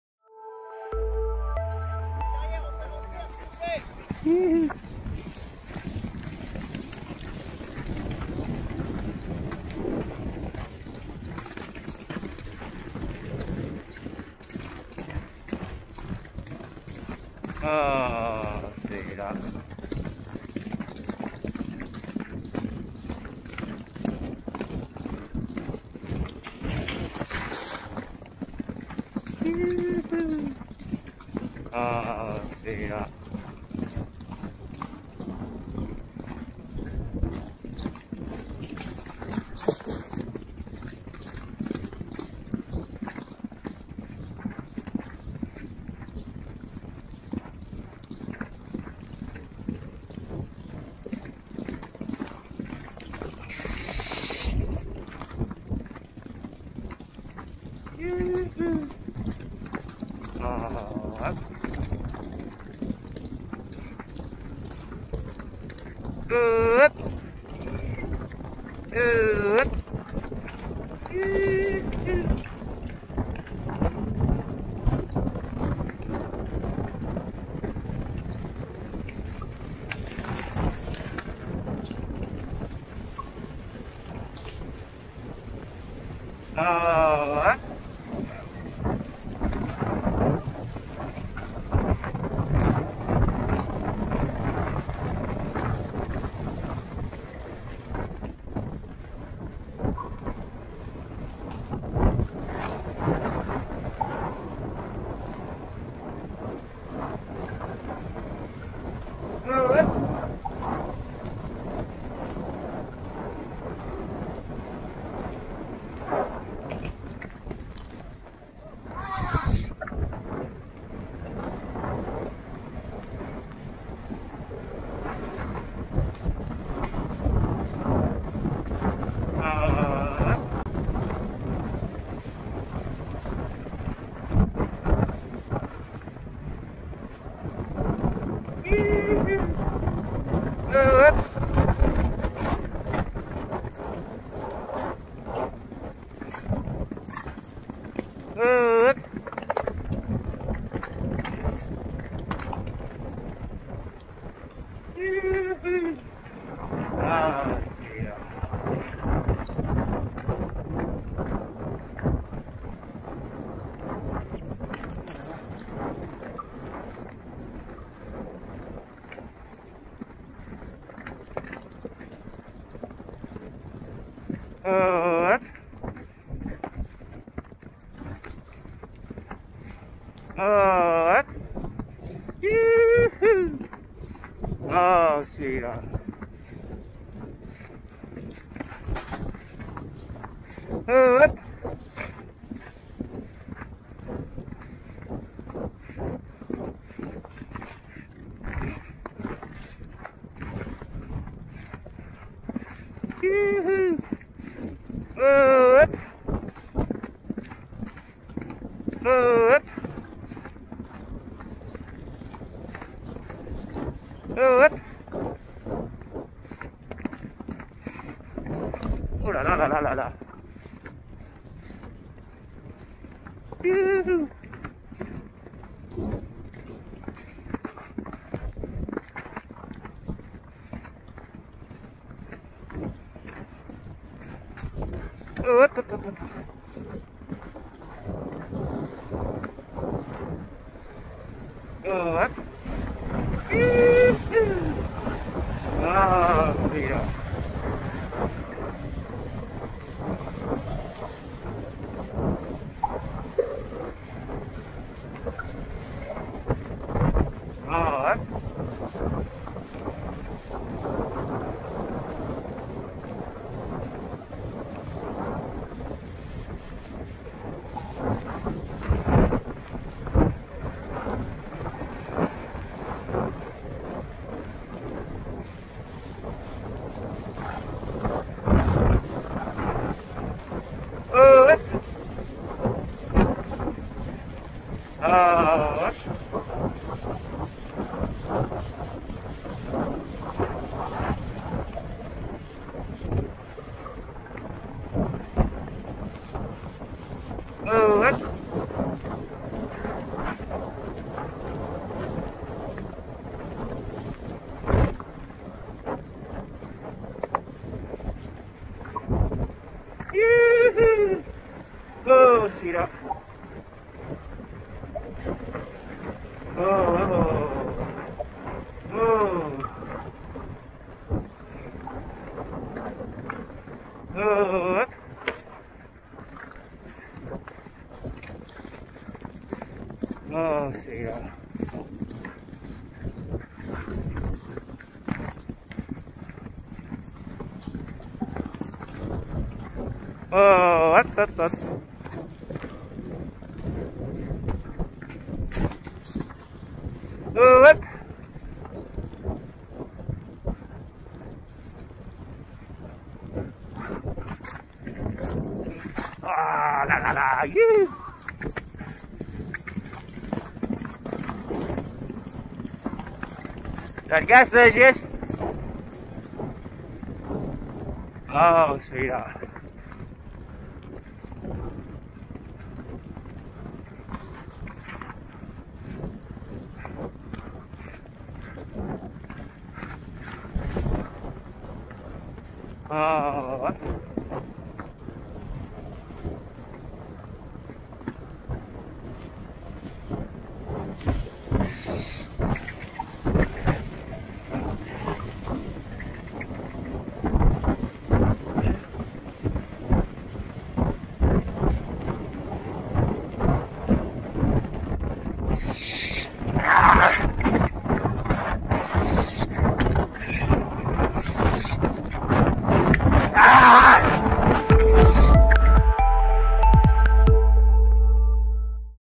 Mettez vous en selle sur Netcam à Pau avec Félix de Giles !
Grand spécialiste du cross, le jockey Félix de Giles vous emmène pour une virée en selle sur la bonne Netcam, 3e aujourd'hui du prix John Henry Wright de Bomari, 1ère étape du Trophée National du Cross à Pau ! Attachez vos ceintures, ça va swinguer !